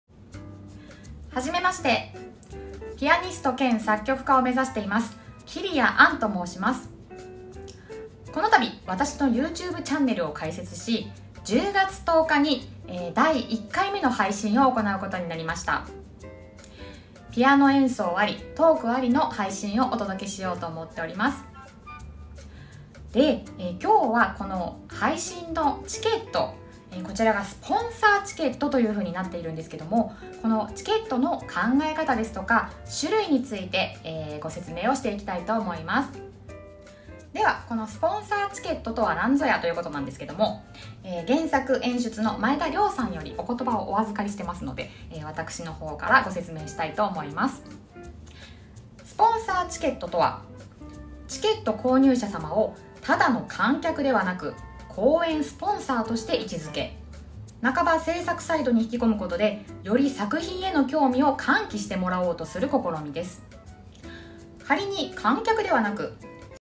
心理音楽劇『I, Rayna 〜 1LDKP』のご案内🎼
榎本のお友達の作曲家兼ピアニスト（のたまご🐣）桐矢杏ちゃんが、皆さんにお伝えしたいことがあるそうなのでシェアします！！😆
YouTubeも開設したそうなので、続きは是非杏ちゃんのYouTubeでご覧ください❤︎

心理音楽劇『I, Rayna 〜 1LDKP』観劇チケットについて
youtu.be/sBv6llRN2Jo

心理音楽劇『I, Rayna 〜 1LDKP』

〜 introduction 〜
不遇のピアニスト・桐矢杏は、初めてのYouTubeライブ配信でも不遇だった。
配信開始と同時に、コメント欄が偉大なクラシック作曲家たちの亡霊に乗っ取られてしまったのだ・・・
（上演時間90分予定）

出演／榎本玲奈

10/10(月祝)　13:30 START（13:00 OPEN）

処：名曲喫茶カデンツァ
大江戸線 本郷三丁目駅より徒歩2分
丸の内線 本郷三丁目駅より徒歩4分

🎫チケット3種
◾️プレミアムチケット ¥8,000
❶会場での観劇（先着25名様予定／全席自由）
❷公演後、スポンサーとしてお名前をクレジットしたお礼動画をお送りします。
❸公演後30日間、ZAIKOでの配信アーカイブを視聴可能。

◾️スペシャルチケット ¥5,000
❶ZAIKOでの配信視聴（公演後3日間アーカイブ視聴可能）

◾️スタンダードチケット ¥3,000
❷公演後、お礼のメールをお送りします。

“スポンサーチケット”の考え方についてお話した動画をYouTubeにもupしておりますので、よろしければそちらもご視聴ください♪

◆チケット販売ページ（ZAIKOにてご購入をお願いいたします）
9/9(金)午前10時 発売開始